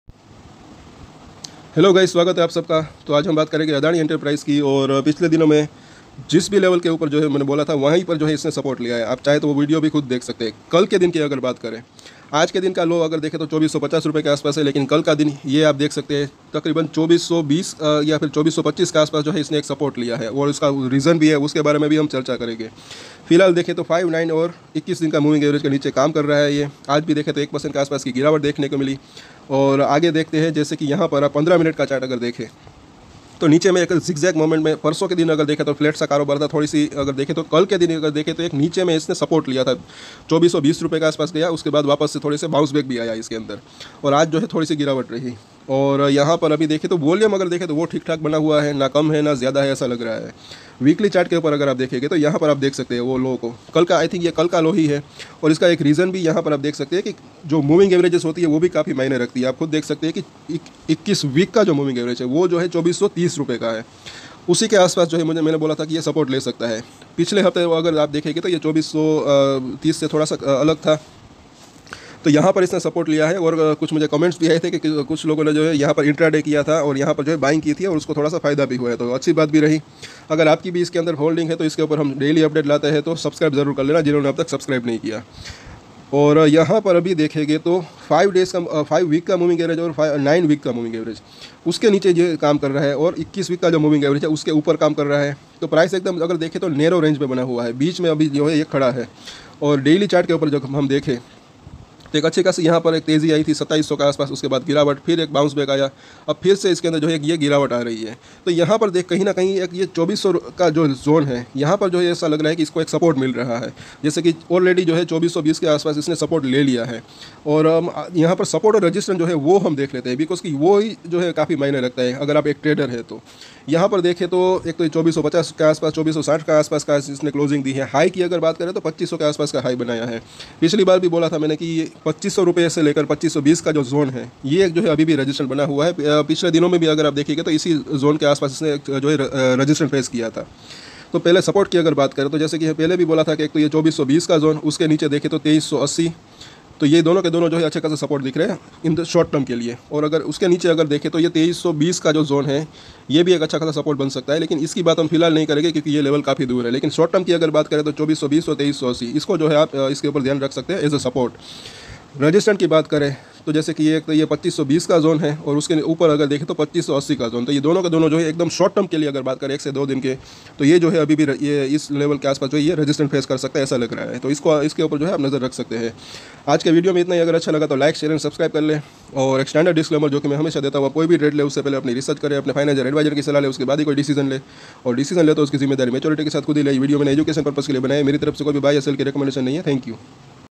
Adani enterprises latest news | adani enterprises share | Vinay Equity
Hello friends in this video I mention about Adani enterprises latest news
adani enterprises share Wednesday 27 September 2023 price analysis
adani enterprises share Wednesday 27 September 2023 price action

Vinay Equity

Thanks